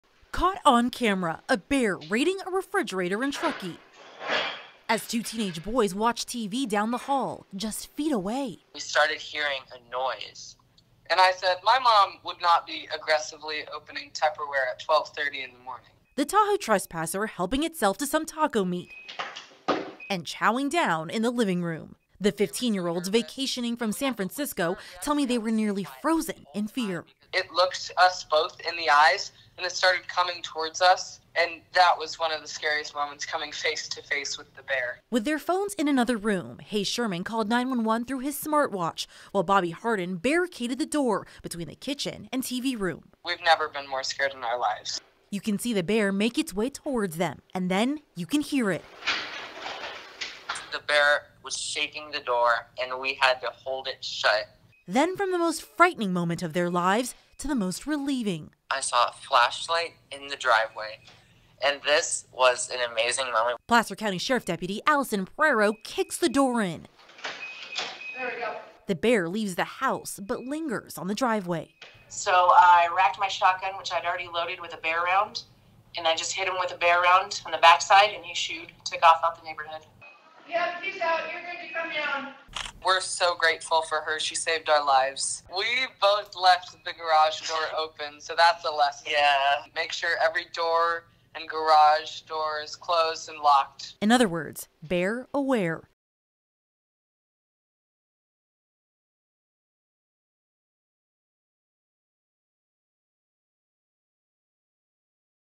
Bear Scare: Teens come face-To-Face with black bear
Two teenage boys from San Francisco said they learned an important lesson after finding a bear inside their vacation home in Truckee. KCRA's Marlei Martinez reports.